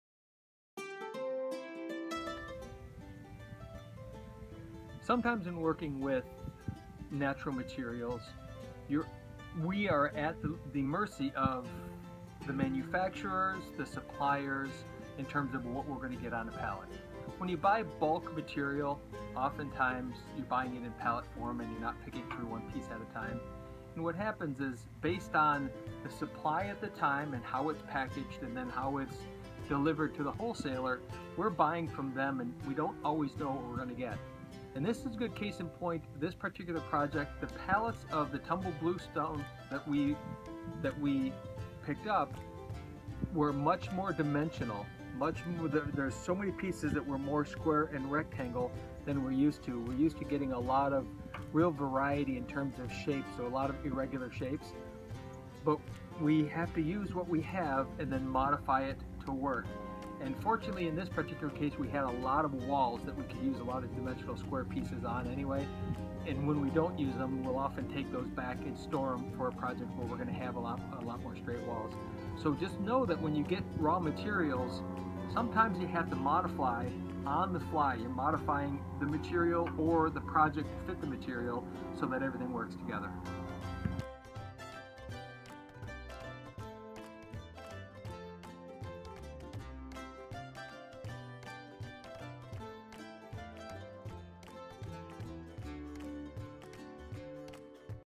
Ask the Pool Guy: Working with Natural Stone Selection {Legendary Escapes}
Working with natural stone looks beautiful but sometimes you have to modify those pieces to all work together.